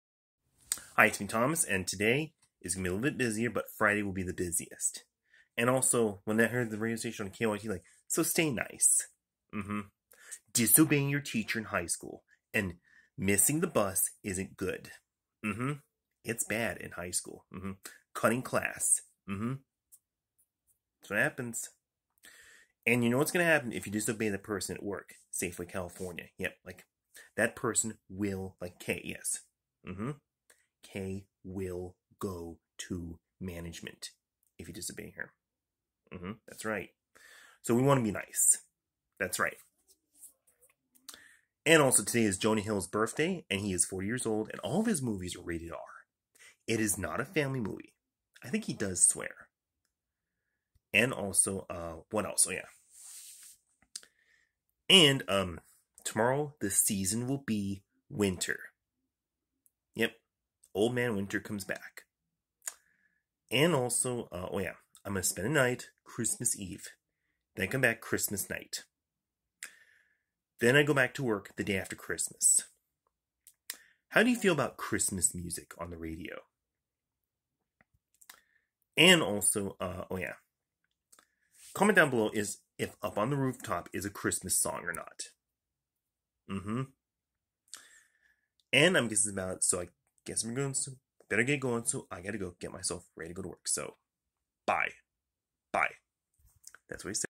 Thomas Murphy knows it is Jonah Hill birthday and all of his movies rated R is not a family movie.
When Thomas heard KOIT Christmas radio and that person said stay nice. It would be bad disobeying your teacher at high school and missing the bus is not good. Cutting class is not good. At Safeway if you disobey your person Kaye will go to management so please listen to the person. Today is Jonah Hill birthday and all of his movies is rated R which is not a family movie. Friday will be the busiest and Thomas will stay the night Christmas eve and he will come back Christmas night. The season will be winter Thursday. Thomas will go out to eat at la pesca blue and it will be slow after Christmas. Next video is December 21.